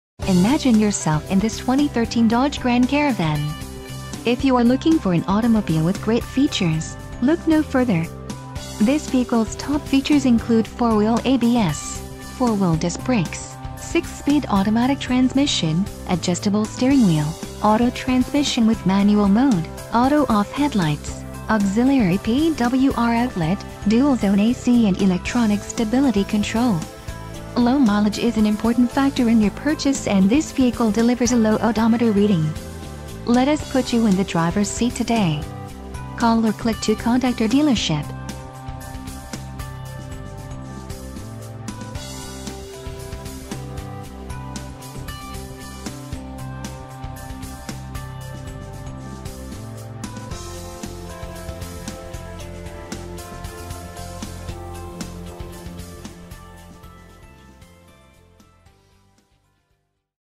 2013 DODGE GRAND CARAVAN 4DR WGN SE - Nimnicht Chevrolet ...
Superb Condition, CARFAX 1-Owner, ONLY 4,229 Miles! $300 below NADA Retail!, FUEL EFFICIENT 25 MPG Hwy/17 MPG City! SE trim. Third Row Seat, Flex Fuel, iPod/MP3 Input, Rear Air, Captains Chairs, Head Airbag. READ MORE! KEY FEATURES INCLUDE: Third Row Seat, Quad Bucket Seats, Rear Air, Flex Fuel, iPod/MP3 Input MP3 Player, Privacy Glass, Keyless Entry, Child Safety Locks, Steering Wheel Controls. SE with Stone White exterior and Black/Light Graystone interior features a V6 Cylinder Engine with 283 HP at 6400 RPM*. Non-Smoker vehicle, Carfax Available. EXPERTS CONCLUDE: "Now back up in the top minivan rankings after a recent update, the Dodge Grand Caravan drives with more precision, has a better interior, and is still hugely practical." -CarAndDriver.com. Great Gas Mileage: 25 MPG Hwy. EXCELLENT VALUE: This Grand Caravan is priced $300 below NADA Retail.